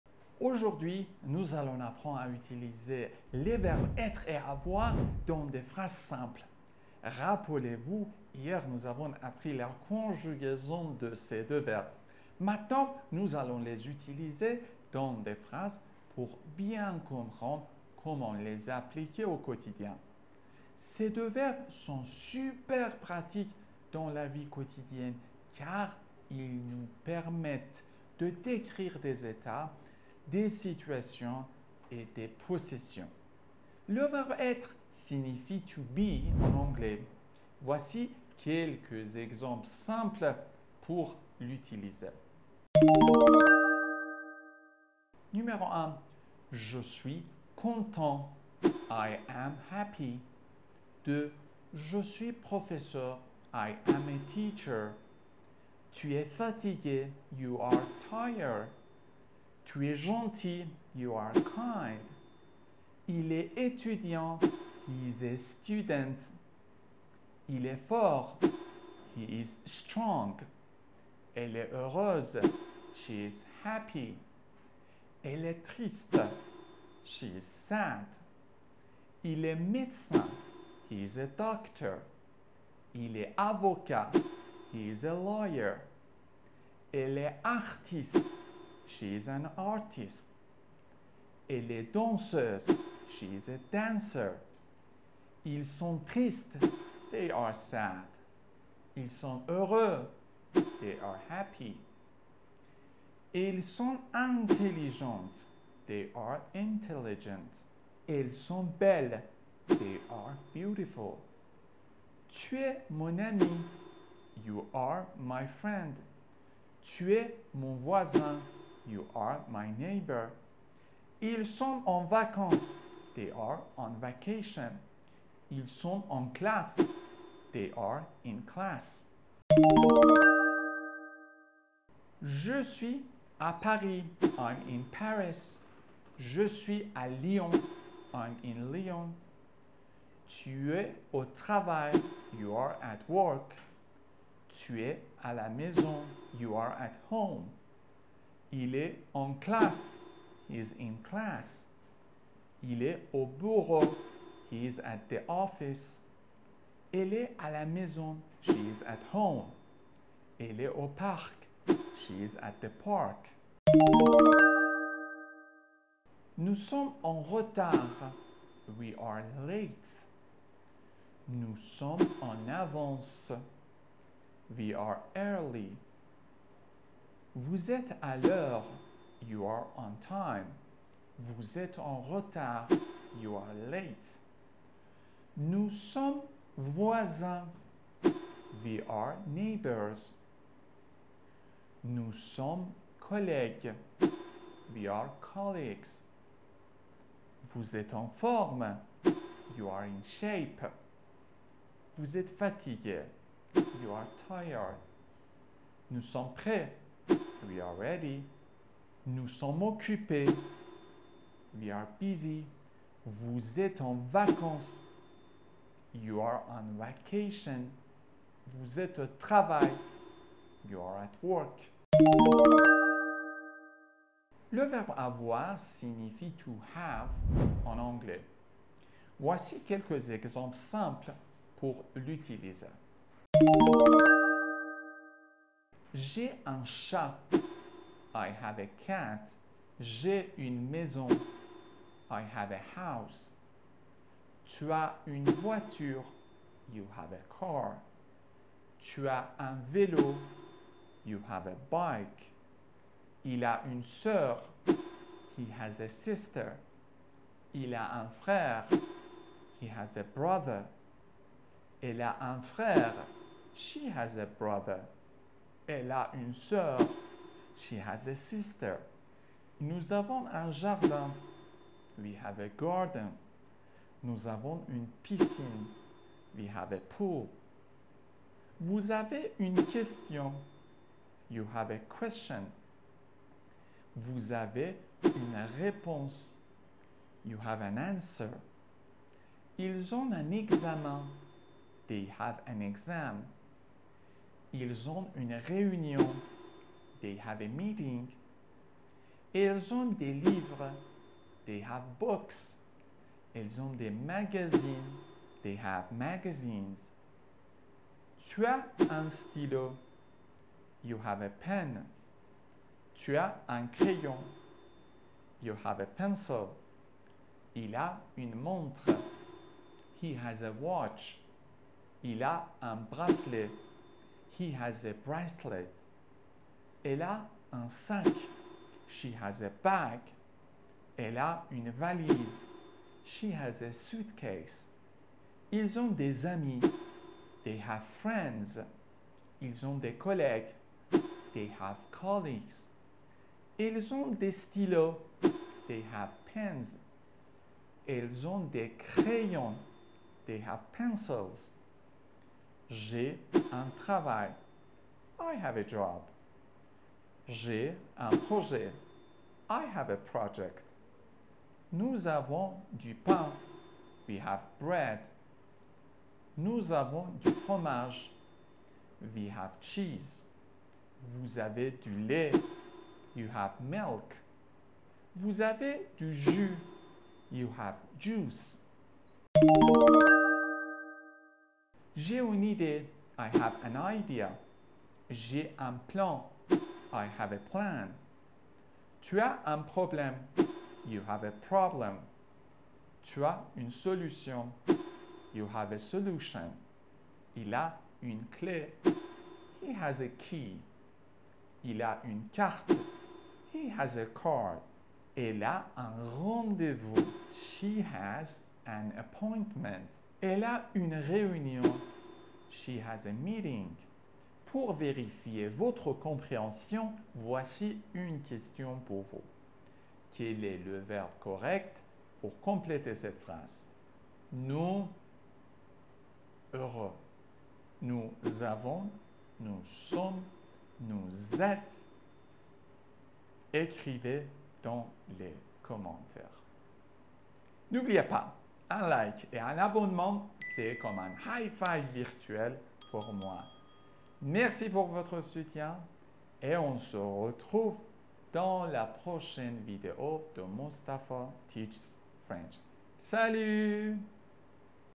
Être et Avoir: 80 Practical Examples - Day 10 of learn French in one month
In this video, you'll discover **80 practical examples** to learn how to conjugate the French verbs **"être"** and **"avoir."** Whether you're a beginner or looking to strengthen your conjugation skills, this video is perfect for you.

The verbs "être" and "avoir" are fundamental in French, whether you're forming compound tenses or expressing states and actions. With these examples, you'll be able to:

- Understand the use of "être" and "avoir" in different contexts.
- Learn how to create simple and complex sentences.
- Improve your pronunciation and fluency in French.

This version is tailored to attract an English-speaking audience interested in learning French grammar.